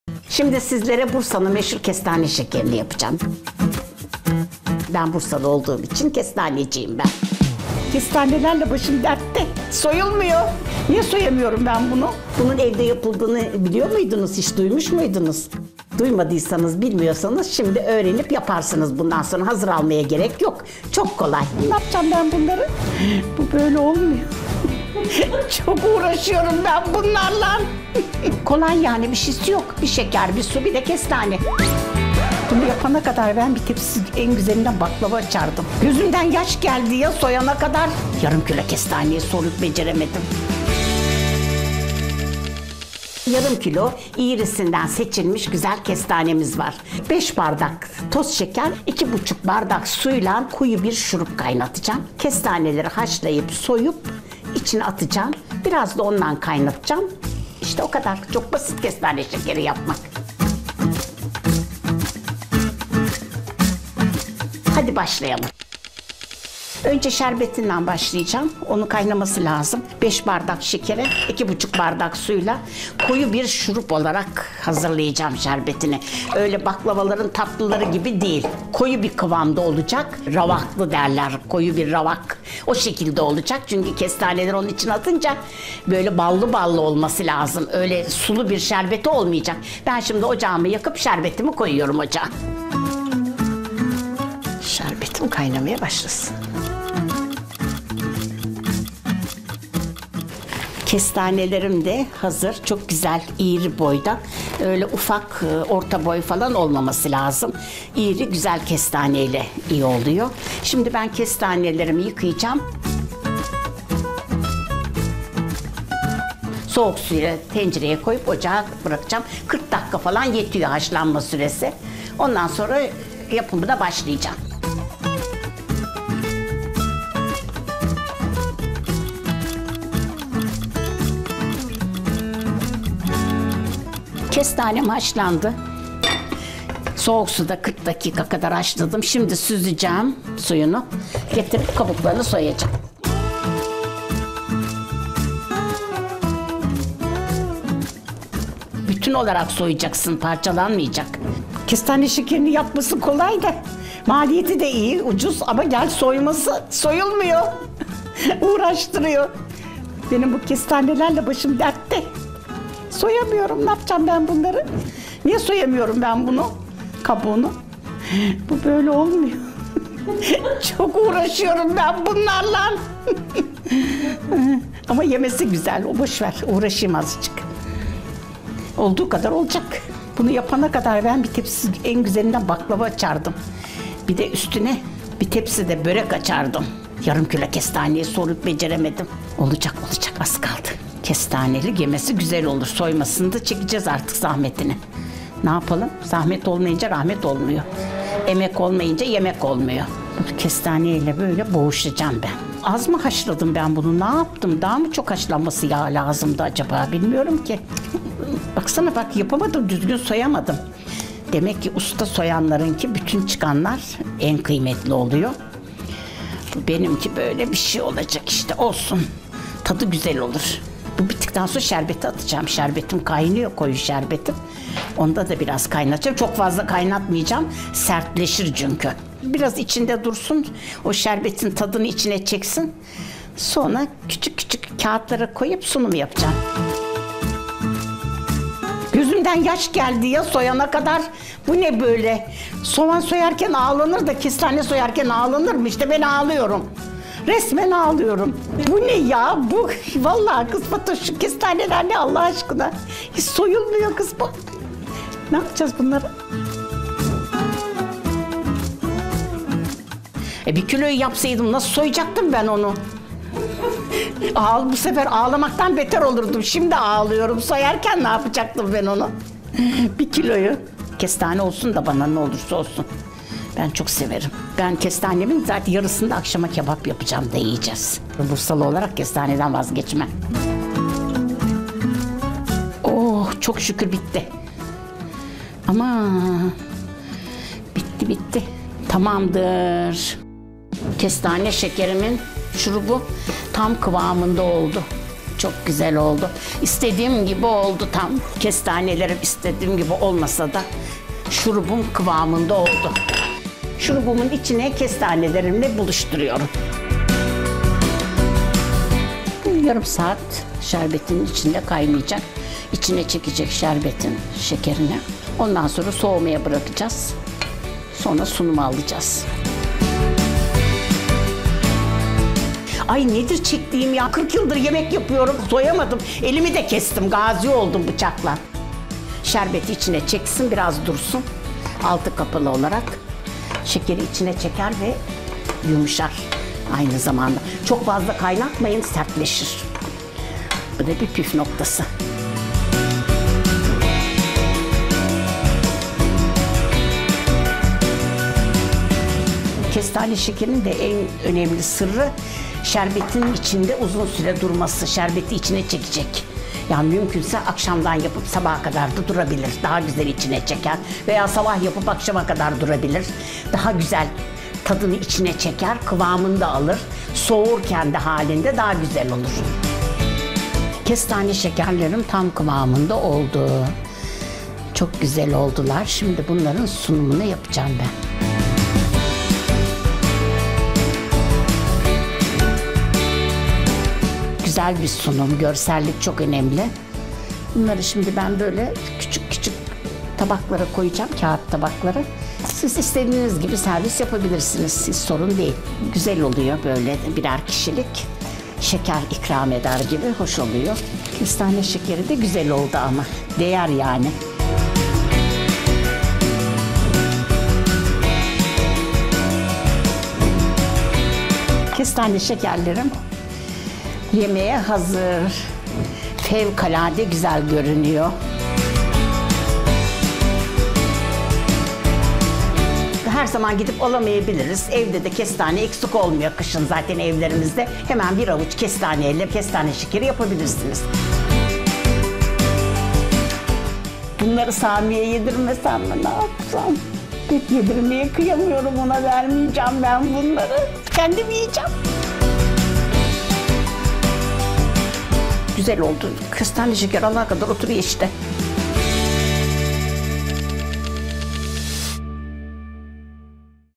Bursa’nın Meşhur Kestane Şekeri Evde Nasıl Kolayca Yapılır? 🌰
Bursalı Ümmü Tunca’nın mutfağına konuk olduk. Lezzetiyle hepimizin beğenisini kazanan Bursa’nın meşhur kestane şekeri tatlısı nasıl yapılır? Evde kolaylıkla yapacağınız kestane şekeri tatlısının yapılışı, püf noktaları ve tarifi videomuzda.

Kolay, hesaplı, lezzetli Bursa’nın meşhur kestane şekerini evde kolayca yapabilmek için malzemeler;

KESTANE ŞEKERİ

1/2 kg iri boy kestane
5 su bardağı şeker
2.5 su bardağı su

ŞURUBU İÇİN:

5 su bardağı şeker
2.5 su bardağı su